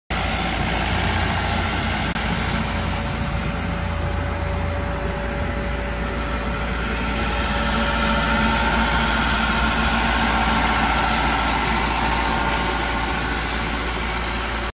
2007 INGERSOLL-RAND XP185WIR For Sale
2007 Ingersoll Rand Air Compressor, 185 CFM, Showing 140 Hours